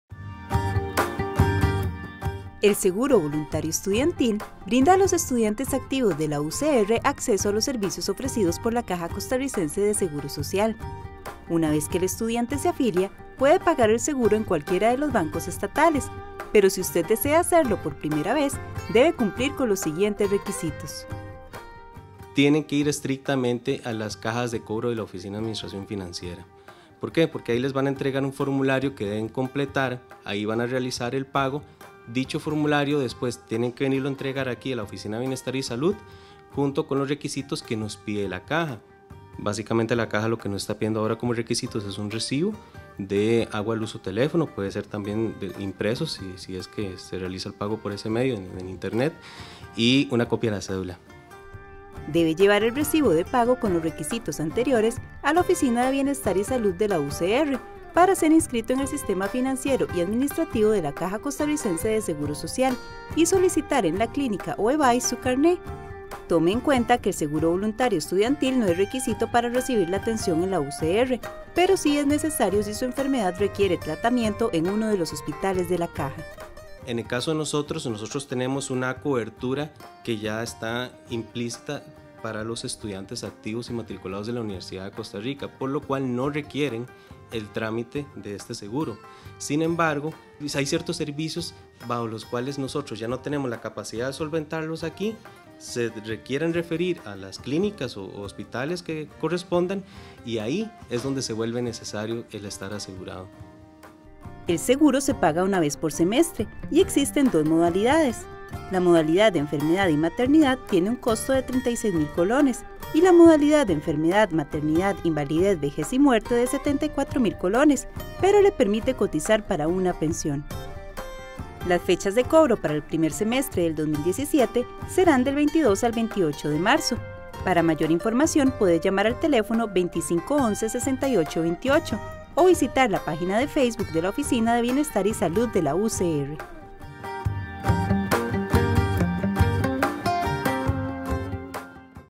Conozca cómo afiliarse al Seguro Voluntario Estudiantil de la UCR
A través de un convenio entre la UCR y la Caja Costarricense de Seguro Social​, los estudiantes de nuestra universidad pueden solicitar un seguro voluntario.